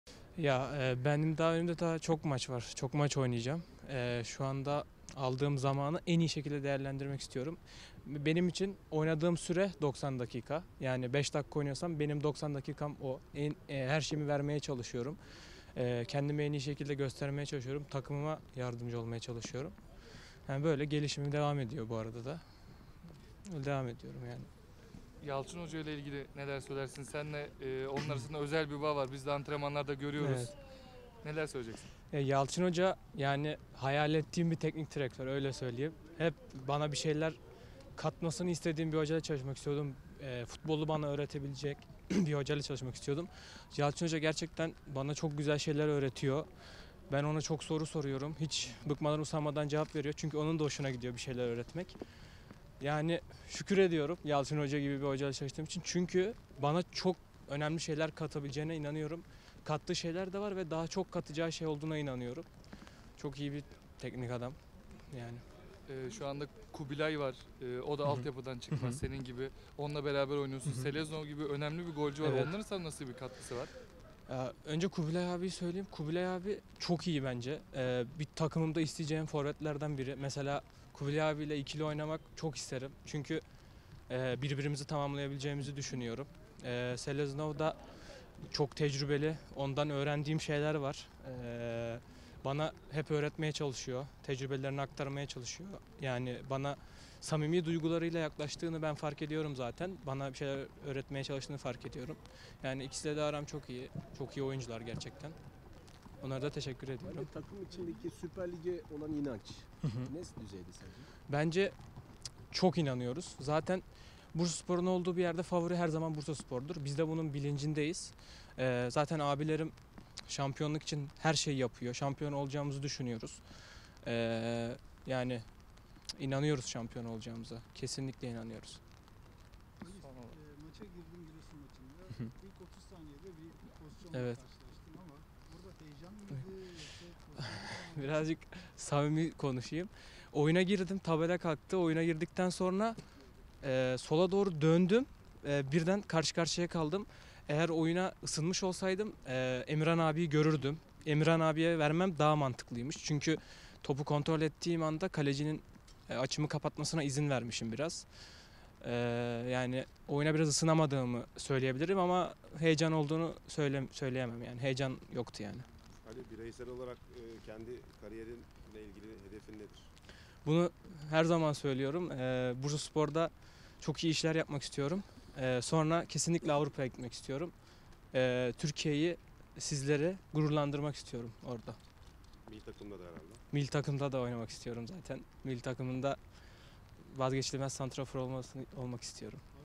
Ali Akman: Yalçın Hoca için şükrediyorum
Bursaspor’un gelecek umudu Ali Akman, Yeşil Beyazlı forma altında başarılı işlere imza atıp Avrupa’ya gitmek istediğini, A Millilerin vazgeçilmez ismi olmayı hedeflediğini söylerken, Yalçın Koşukavak ile çalıştığı için şükrettiğini belirtti.